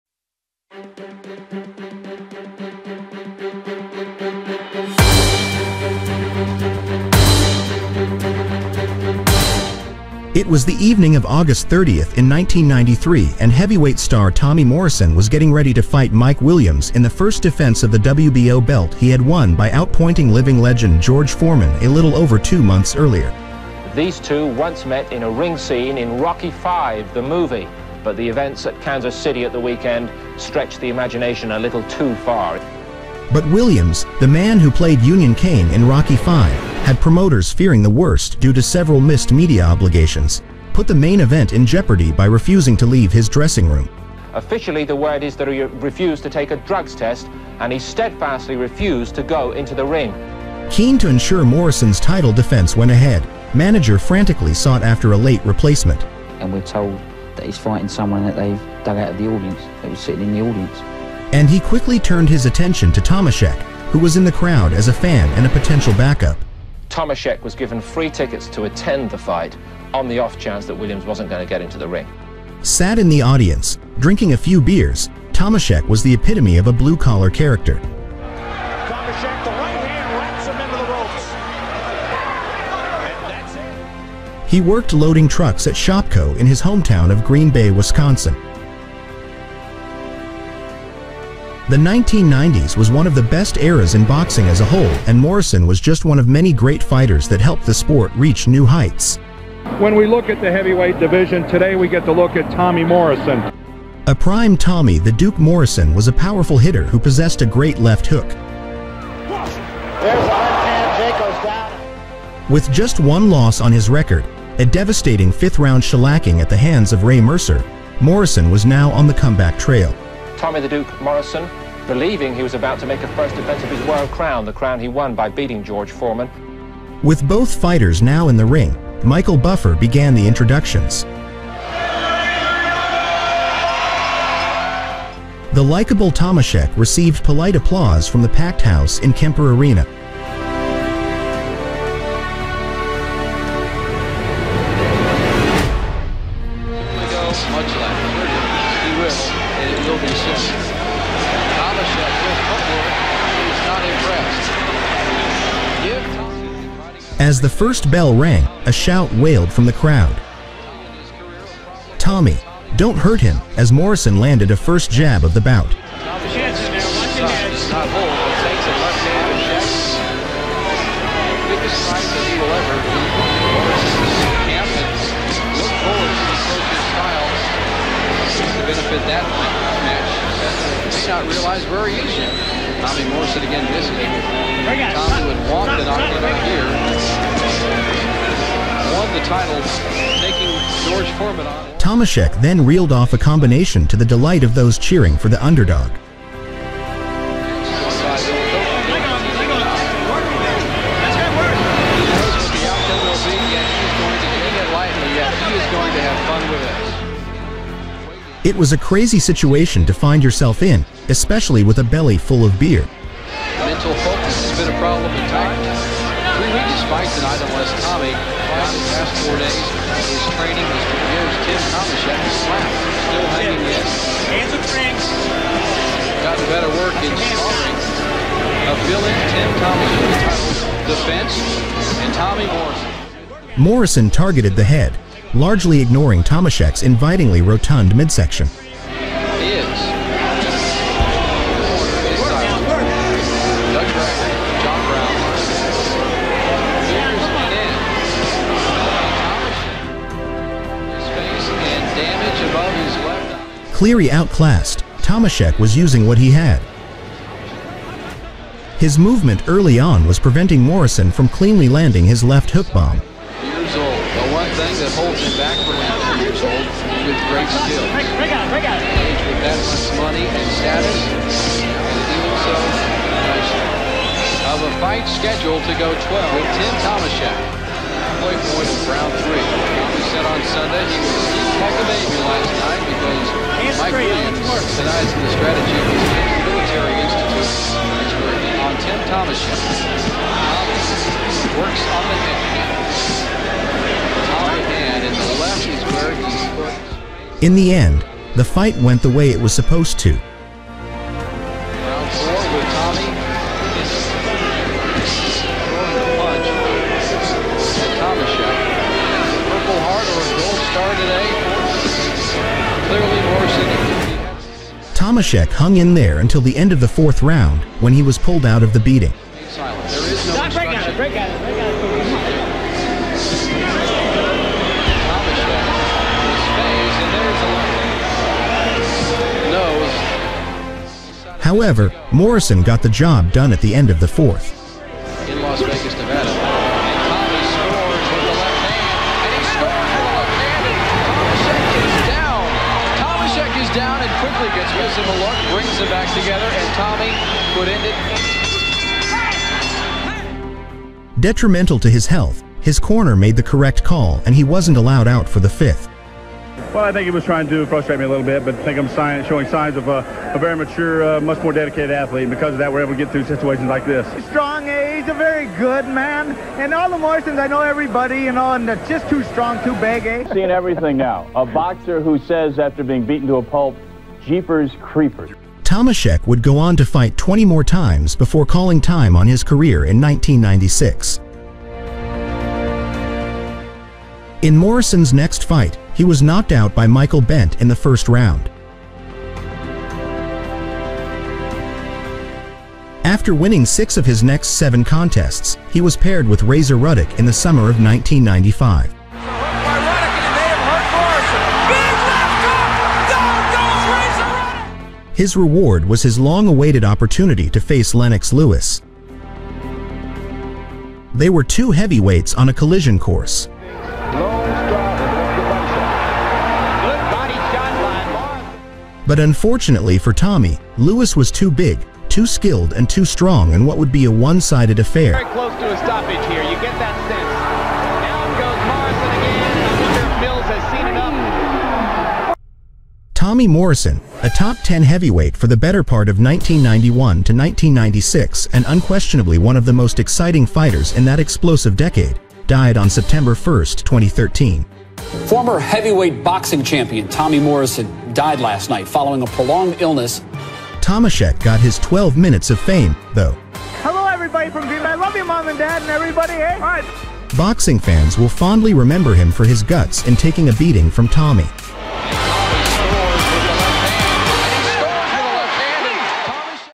When Tommy Morrison Met A Drunk Man From The Crowd
Tim “The Doughboy” Tomashek (35-10) challenged WBO heavyweight champion of the world Tommy “The Duke” Morrison (38-1) for the WBO heavyweight title.

Venue: Kemper Arena, Kansas City, MO, USA